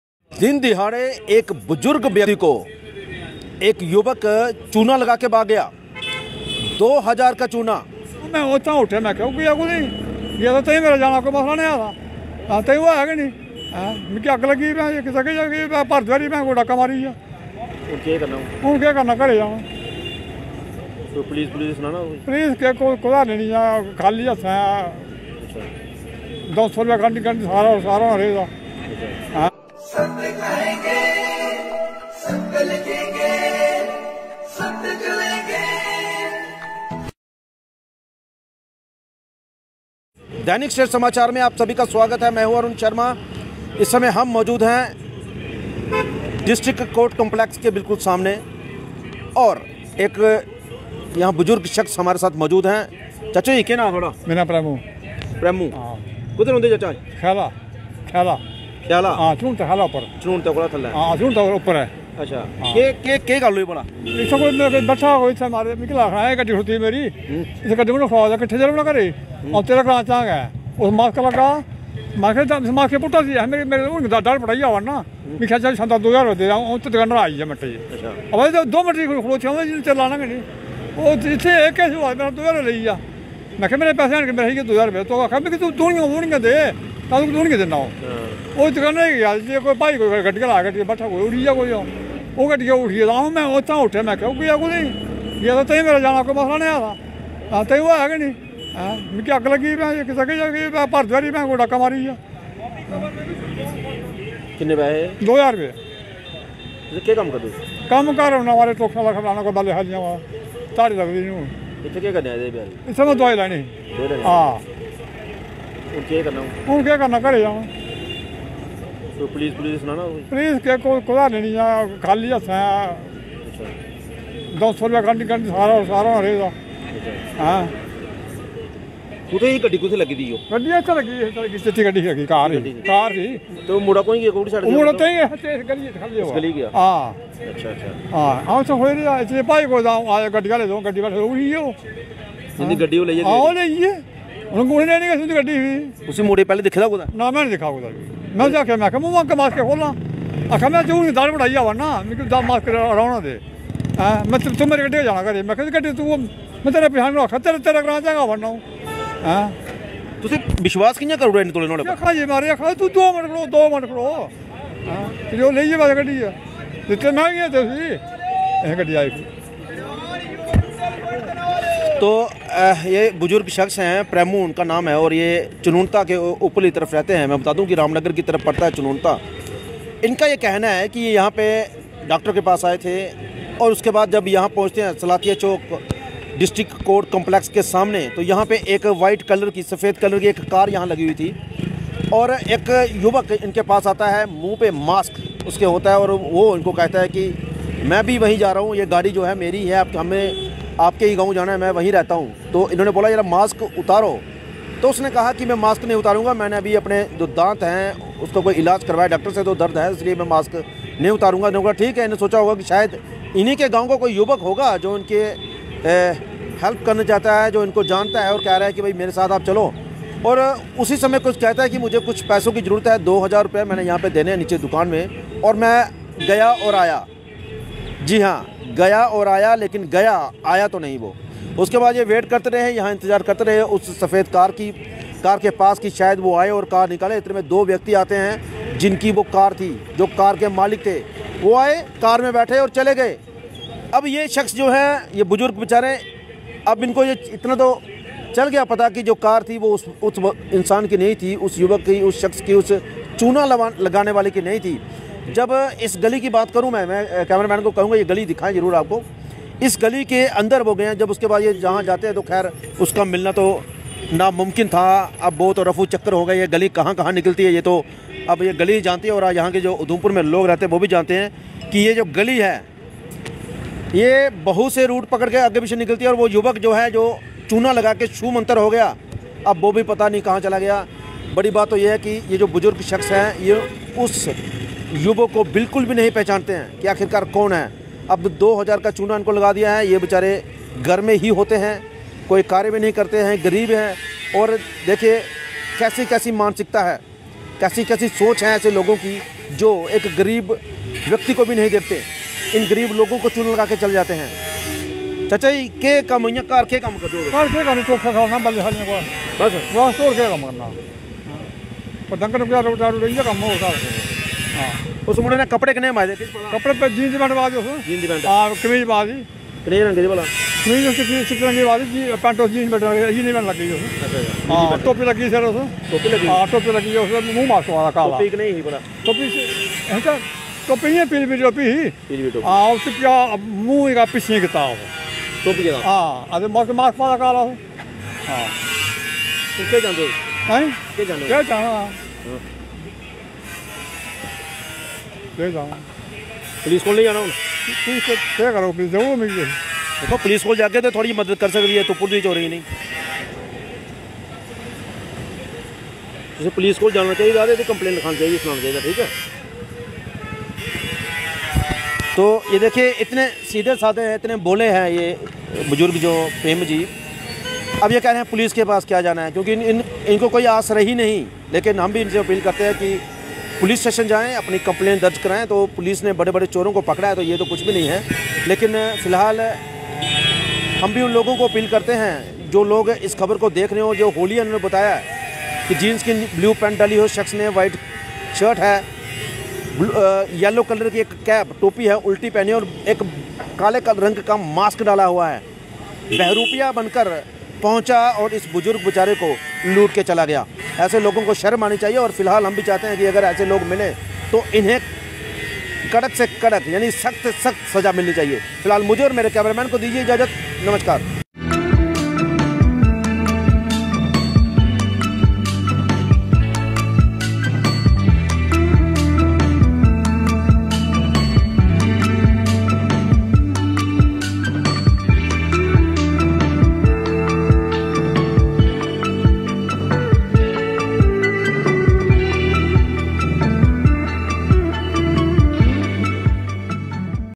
शर्मनाक दिनदहाड़े एक गरीब व्यक्ति को चुना लगाकर ₹2000/- लेकर भागा एक युवक
शर्मनाक दिनदहाड़े एक गरीब व्यक्ति को चुना लगाकर ₹2000/- लेकर भागा एक युवक. Udhampur के Distt. Court Complex के सामने का है यह मामला, घर से दवाई लेने आया था प्रेम नाथ नाम का एक बुजुर्ग शख्स और एक लुटेरा व्यक्ति 2000 का चूना लगा कर भाग गया ll
for_help PREM NATH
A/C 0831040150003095
IFSC : JAKA0DALSAR